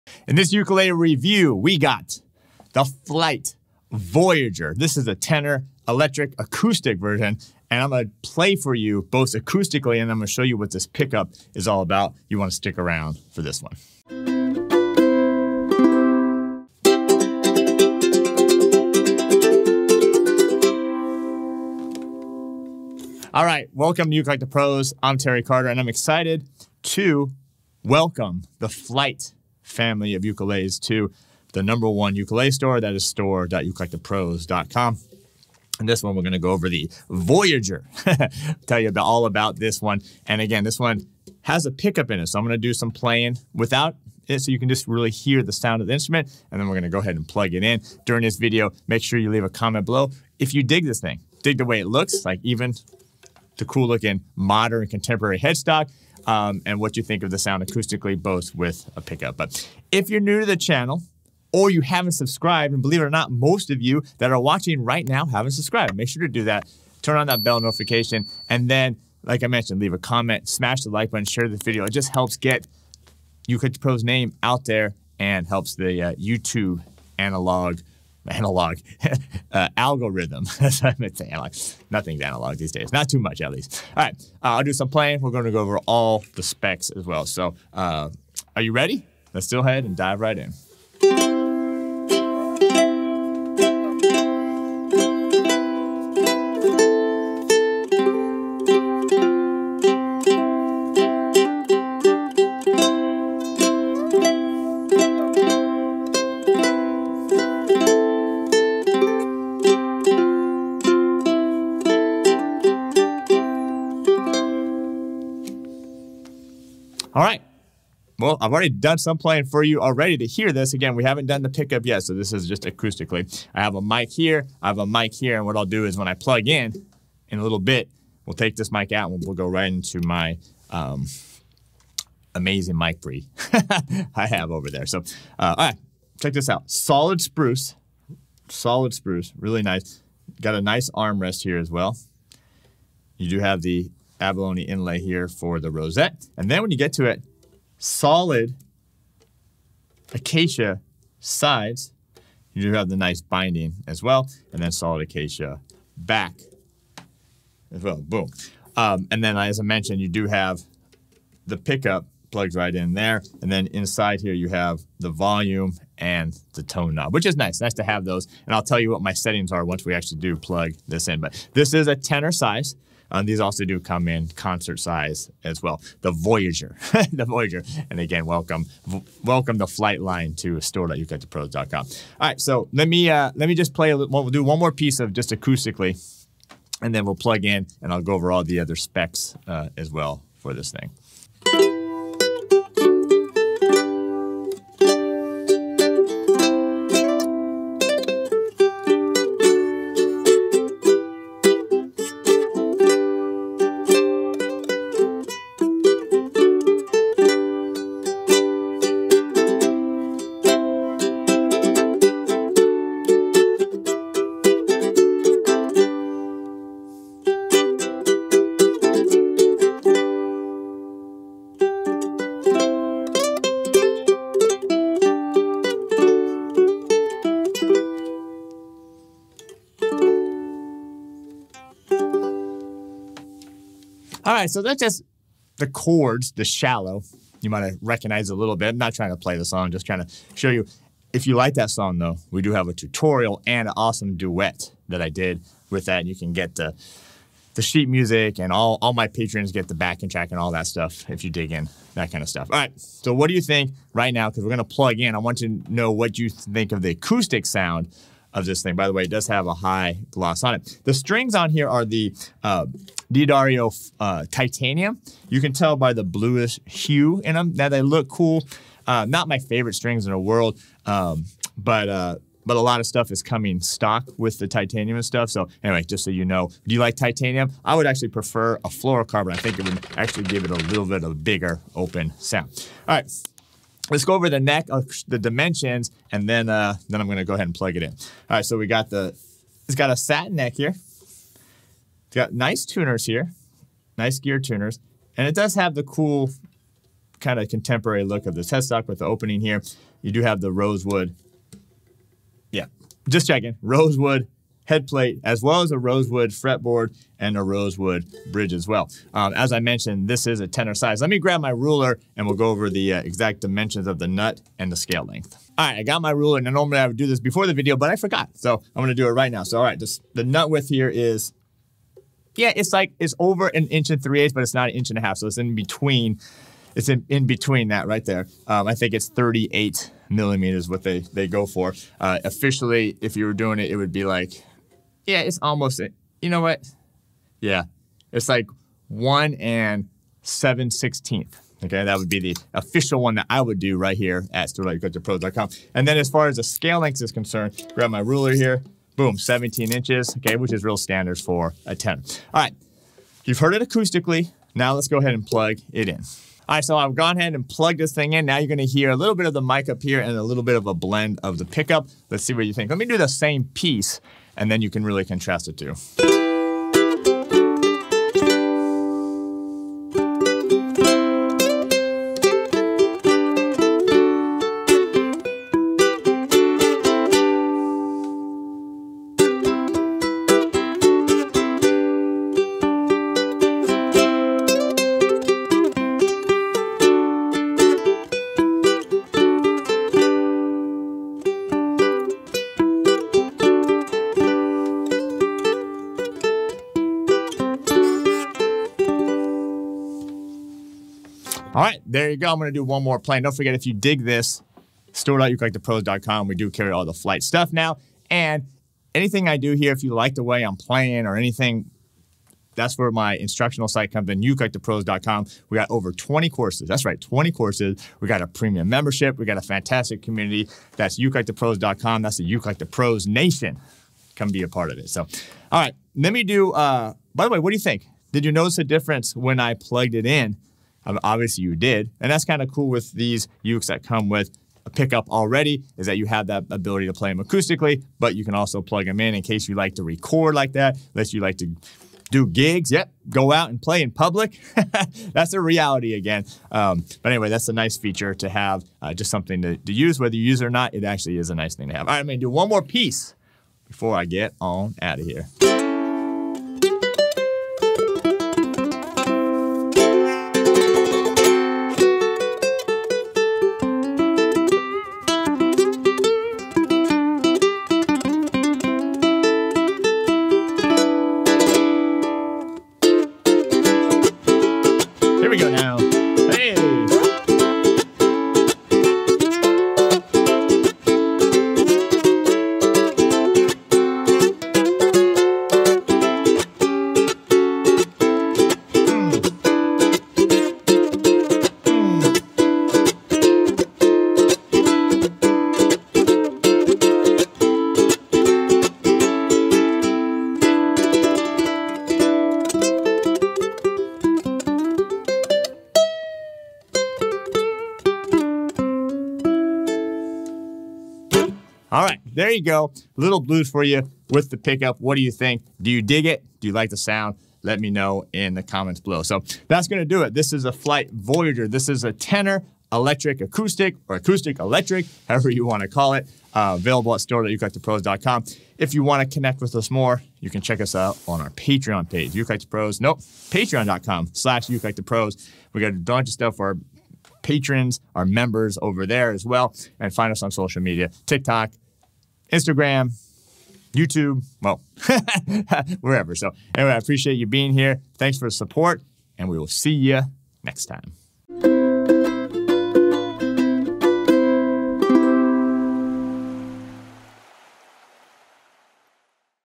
Flight Voyager Electro-Acoustic Tenor Ukulele | Ukulele Review
Hey guys in this new ukulele review video I'm showing the Flight Voyager Electro-Acoustic Tenor Ukulele Royal Series!

The Voyager uses a highest grade solid spruce top with solid acacia back and sides.  Spruce doesn’t have any of the wild and beautiful patterns of other exotic woods but it is an unassuming stalwart for some of the richest tones you can get on a solid wood instrument. Married with acacia for the back and sides, we have used a perfect combination of these woods that is favored by some of the worlds best luthiers.

00:00 Intro
01:33 Sound check without Pickup
02:17 Specs
03:11 Sound check without Pickup
04:04 Shallow Tutorial
04:12 More Specs
06:06 Sound check with Pickup
07:35 Sound check with Pickup

If you are interested in this cool instrument go and check them now

4) READY TO BUY A UKULELE?

6) ARE YOU INTERESTED IN GUITAR?

Terry